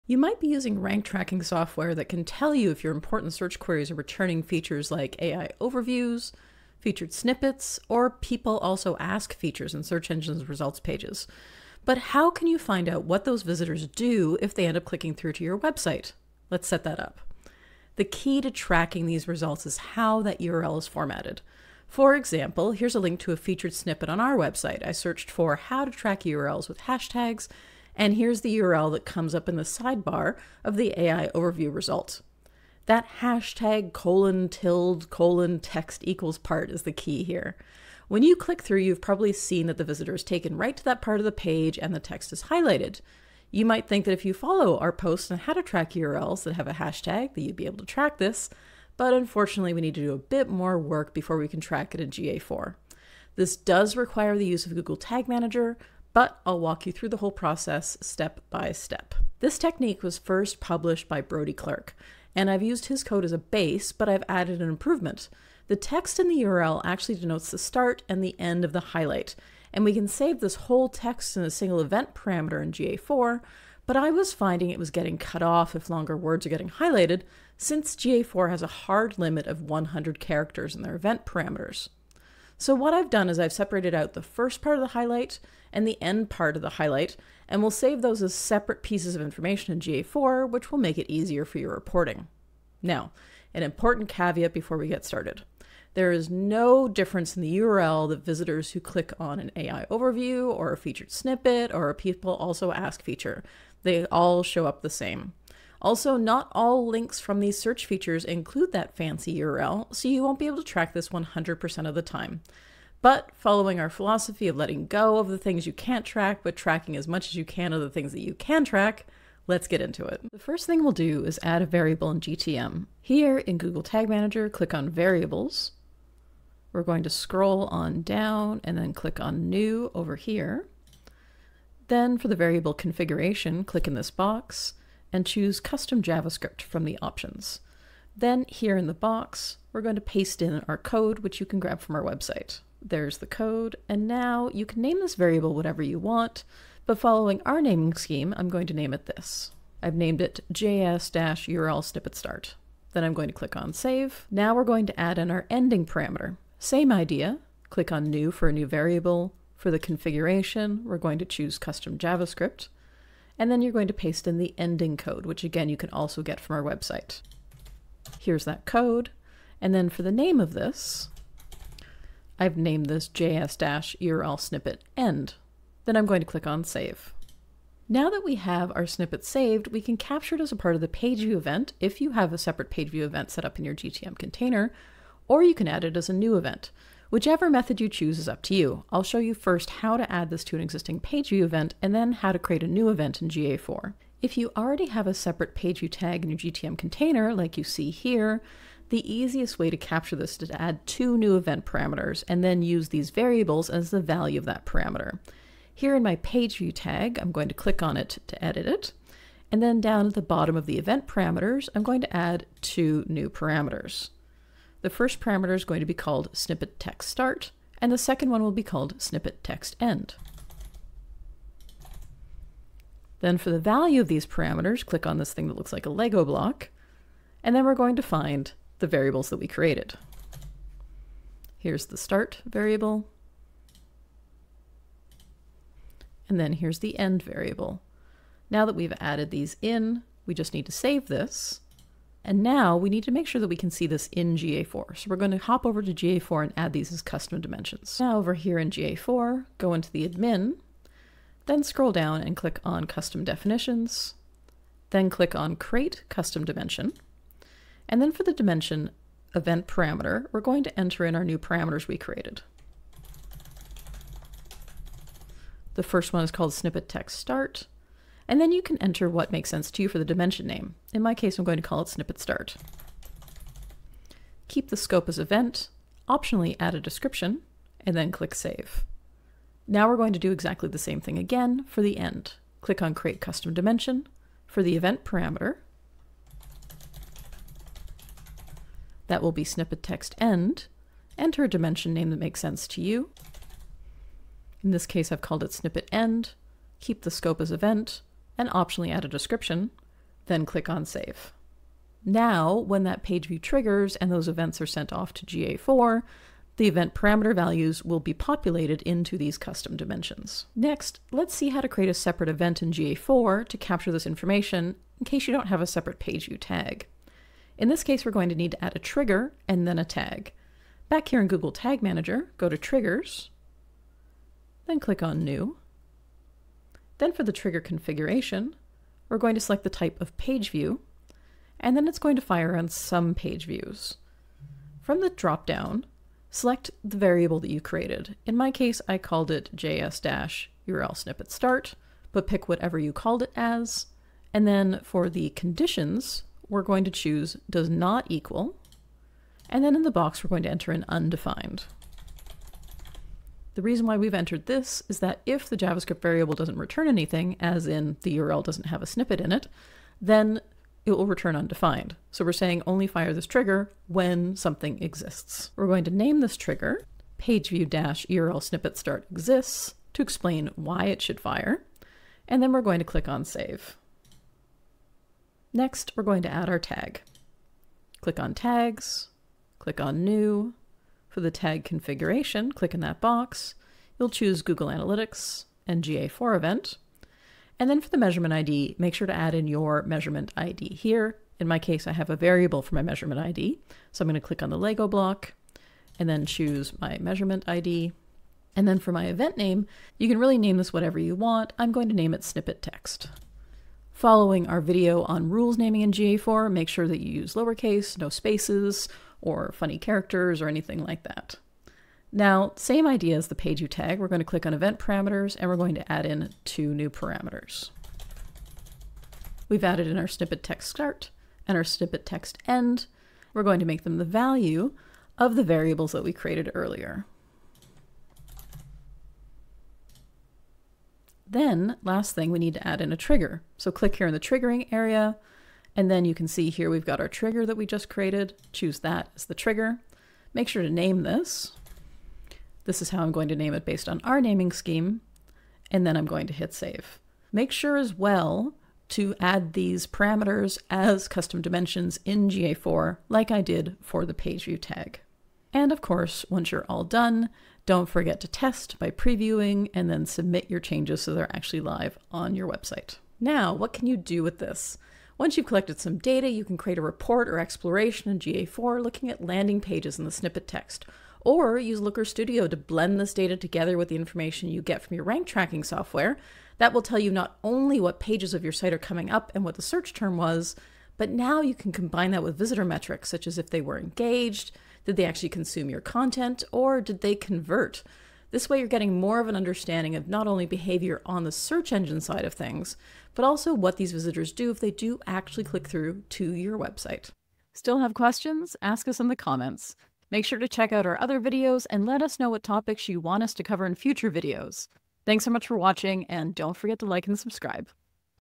How to Track Traffic from AI Overviews, Featured Snippets, or People Also Ask Results in GA4
Ever wonder if Google’s AI Overviews, Featured Snippets, or People Also Ask (PAA) results are sending traffic to your website? 🤔 Unfortunately, GA4 doesn’t track these clicks by default—but we’ve got you covered.

In this video, Dana DiTomaso walks you through a step-by-step Google Tag Manager (GTM) and GA4 setup to track visits from these AIO, Featured Snippets, and PAA results. You’ll learn how to extract snippet highlights, set up event tracking, so you can analyze user behavior.

✅ How to track AI Overview, Featured Snippet & PAA clicks in GA4
✅ The key to extracting snippet highlights from URLs
✅ How to set up event parameters in Google Tag Manager
✅ The best way to analyze this data in GA4 & Looker Studio

=== PREREQUISITES ===
📌 A basic understanding of GA4 & Google Tag Manager
📌 Access to your GA4 property and GTM container
📌 A rank tracking tool (optional, but helpful)

=== TIMESTAMPS ===
0:00 Intro: Why track AIO, Featured Snippets & PAA?
1:15 How Google formats URLs for AIO, Snippets & PAA
3:00 Setting up variables in Google Tag Manager
5:45 Adding custom dimensions in GA4
7:30 Creating an event to capture snippet text
9:00 How to analyze and report on this data
10:30 Troubleshooting common issues

=== TROUBLESHOOTING TIPS ===
⚡ Not seeing data in GA4? Double-check your event parameters.
⚡ Snippet text isn’t captured properly? Ensure your GTM variables are set up correctly.
⚡ Still not seeing data in GA4? Double-check your custom dimensions.

=== STAY AHEAD WITH KP PLAYBOOK’S NEWSLETTER ===